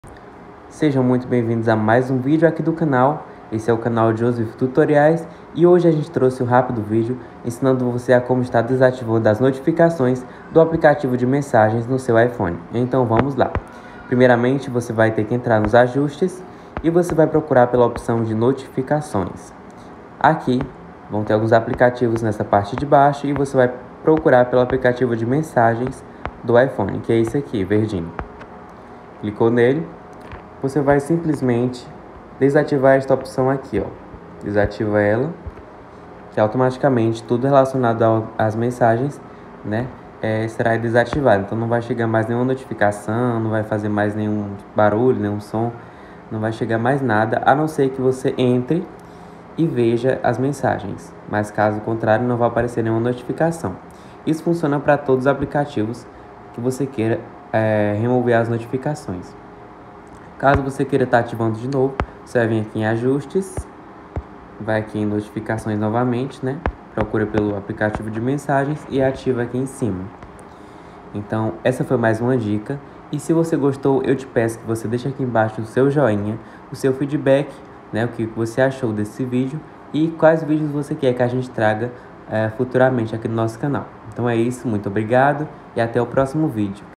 Como ATIVAR a ECONOMIA DE DADOS no IPHONE!
Fala galerinha que assiste o nosso canal. Beleza? Neste vídeo de hoje, viemos ensinar você a como economizar dados através da ativação de uma função muito boa no seu iphone.Confira!